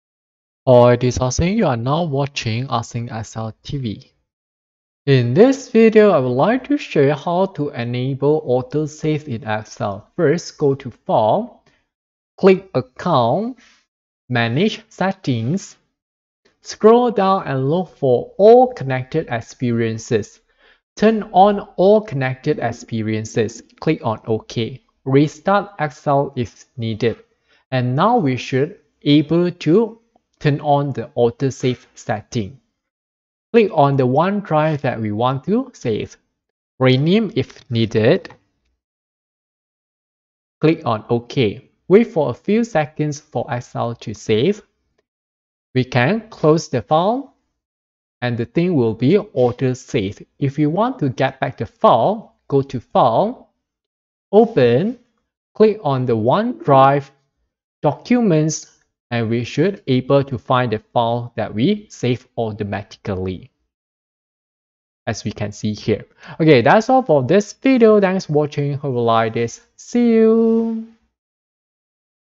Enable autosave in excel
Unable to turn on the AutoSave button in MS Excel 365? Change this privacy setting and we should be able to turn it on.
How to enable AutoSave in Microsoft Office Excel 365?